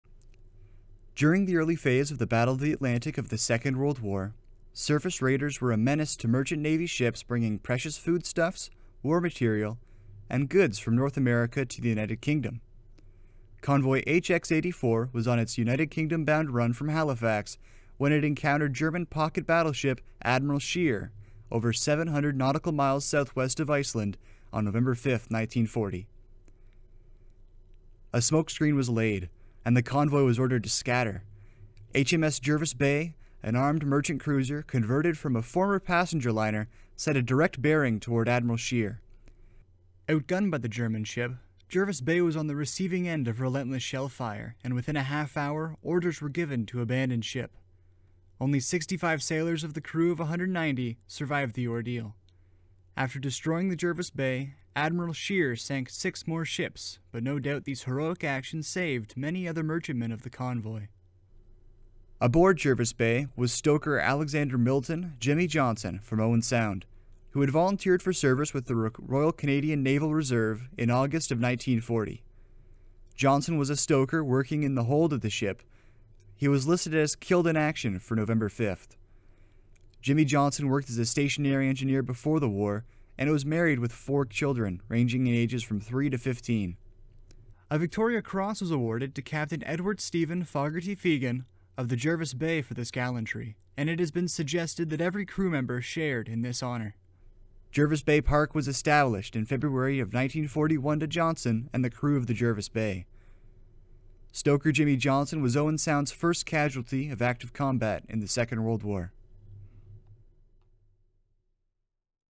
6 MHT Jervis Bay Park
Jervis Bay Park to commemorate HMS Jervis Bay and those who served on her.